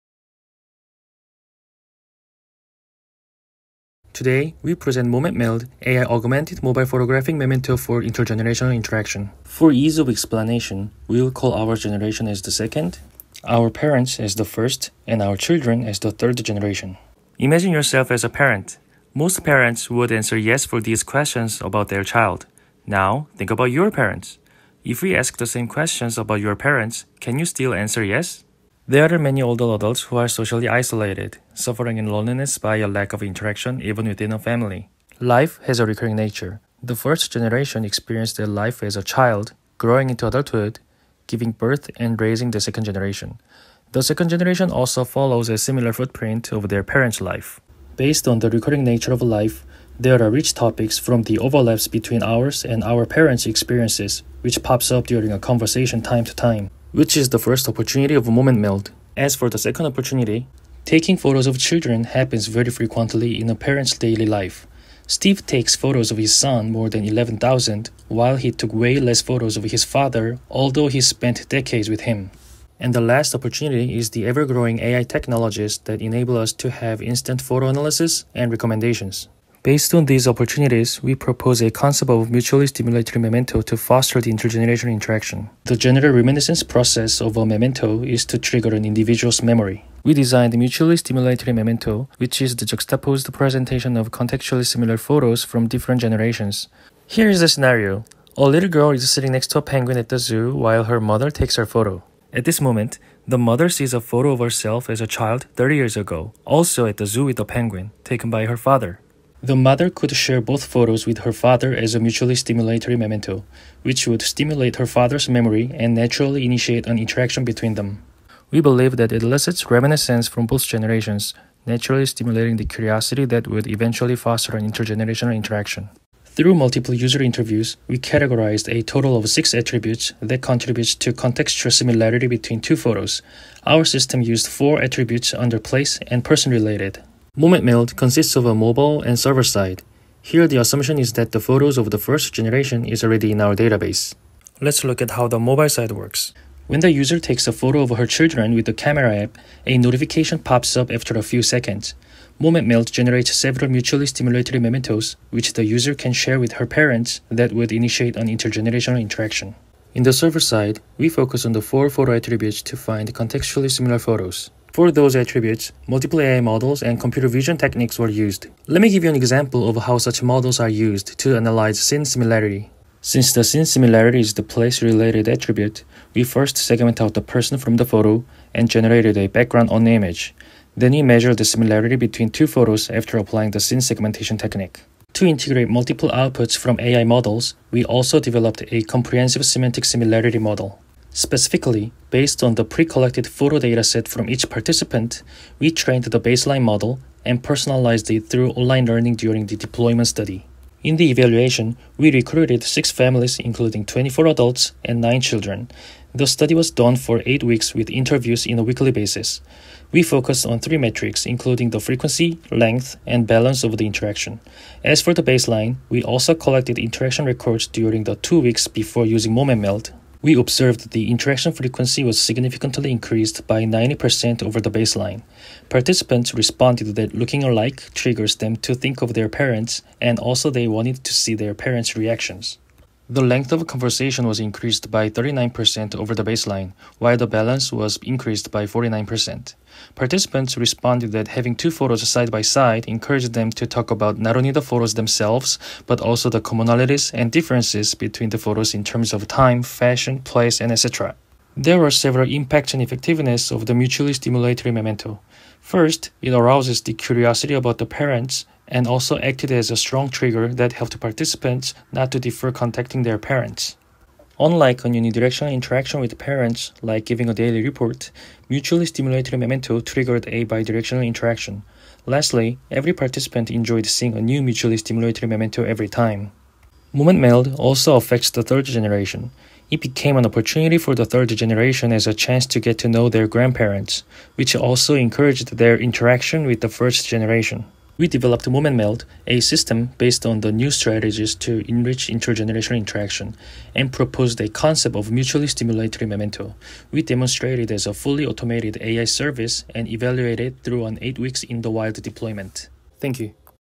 MomentMeld: AI-augmented Mobile Photographic Memento towards Mutually Stimulatory ...
MomentMeld: AI-augmented Mobile Photographic Memento towards Mutually Stimulatory Inter-generational Interaction
Bumsoo Kang, Seungwoo Kang, Inseok Hwang

Session: Tech for Learning and Families

Abstract
Aging often comes with declining social interaction, a known adversarial factor impacting the life satisfaction of senior population. Such decline appears even in family--a permanent social circle, as their adult children eventually go independent. We present MomentMeld, an AI-powered, cloud-backed mobile application that blends with everyday routine and naturally encourages rich and frequent inter-generational interactions in a family, especially those between the senior generation and their adult children. Firstly, we design a photographic interaction aid called mutually stimulatory memento, which is a cross-generational juxtaposition of semantically related photos to bring natural arousal of context-specific inter-generational empathy and reminiscence. Secondly, we build comprehensive ensemble AI models consisting of various deep neural networks and a runtime system that automates the creation of mutually stimulatory memento on top of the user's usual photo-taking routines. We deploy MomentMeld in-the-wild with six families for an eight-week period, and discuss the key findings and further implications.